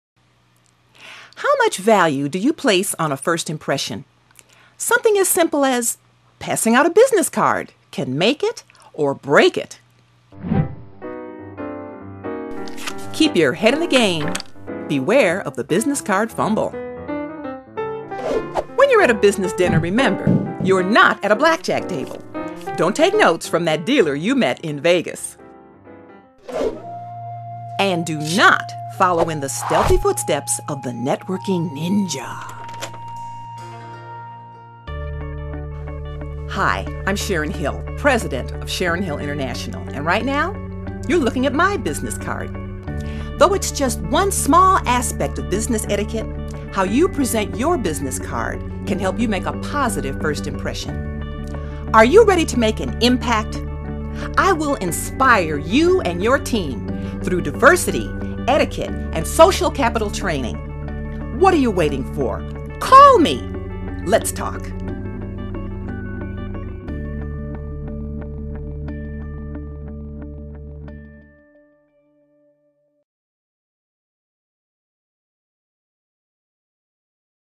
Sharon Hill Pumps Up Your Professionalism
Certified American business etiquette trainer teaches students and professionals how to make positive, lasting first impressions.